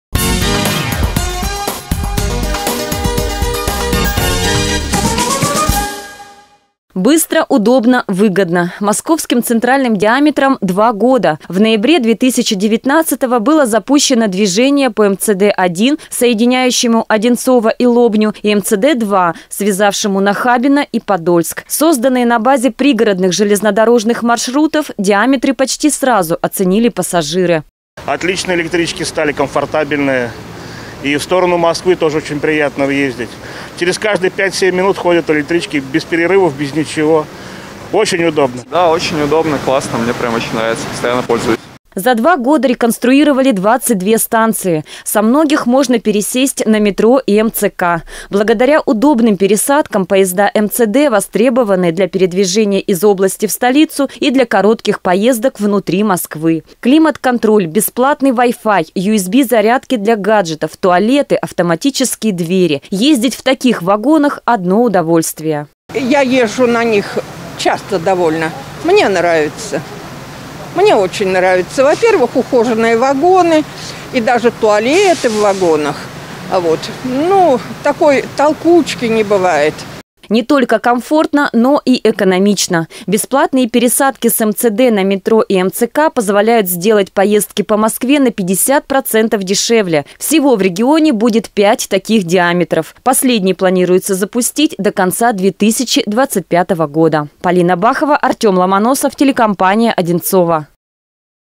Два года МЦД
От устаревших электричек к современному наземному метро. Два года назад было запущено движение по Московским центральным диаметрам.